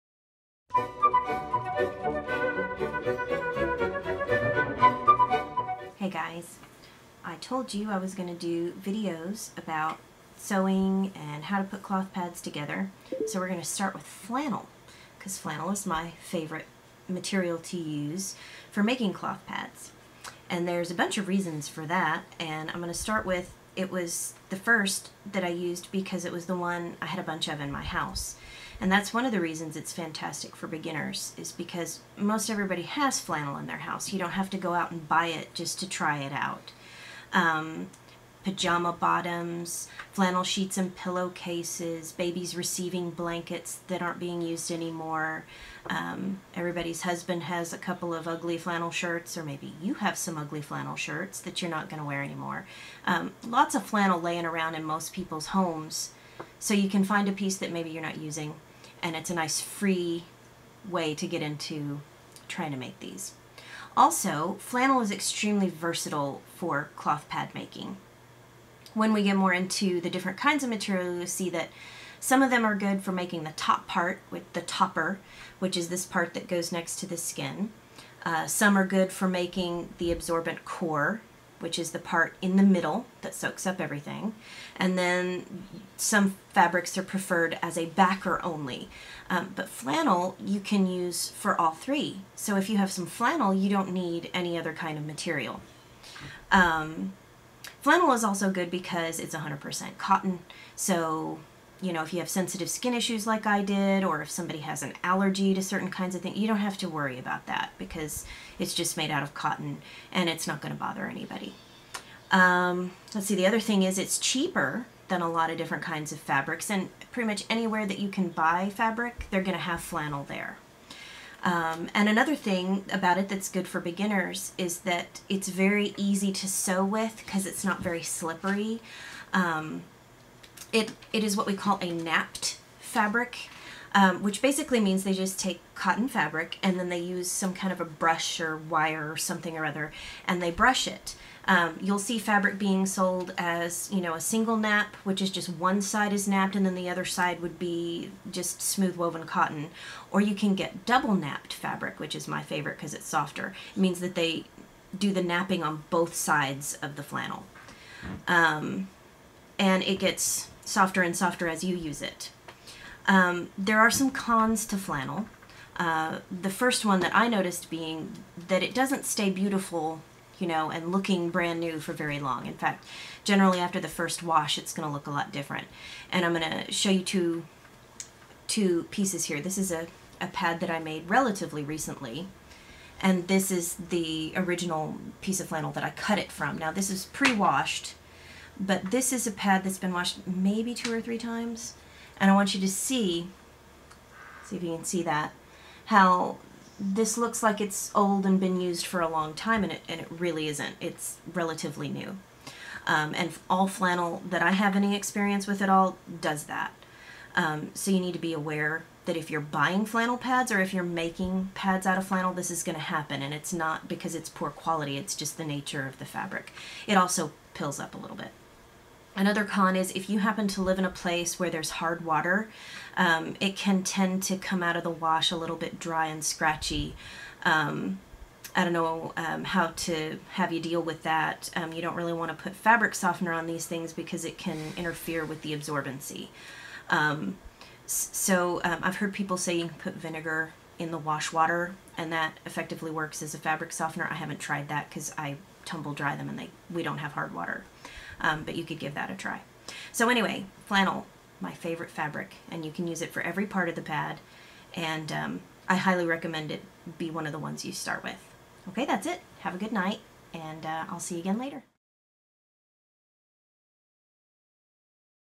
Sewing Cloth Pads 101 - Flannel!
This is a video about using flannel fabric when you sew your own cloth menstrual pads.  It discusses the pros and cons of flannel as well as its versatility of use in different parts of the cloth pad.

Also let me know what other sewing videos you would like to see!